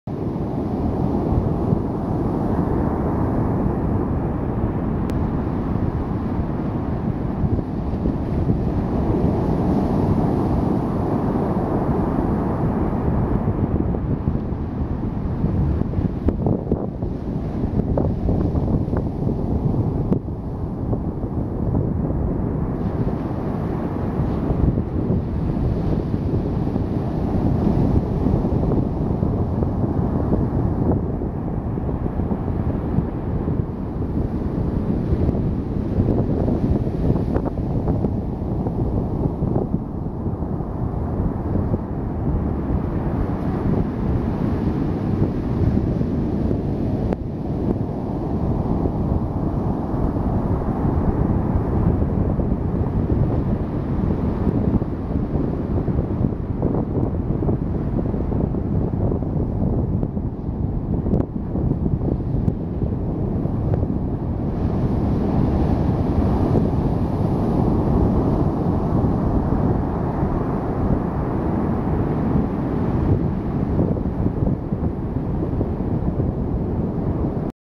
Winter Surfer
Dude, on a surfboard, waiting to catch the perfect winter wave at Robert Moses State Park, Long Island, New York (February 4, 2016).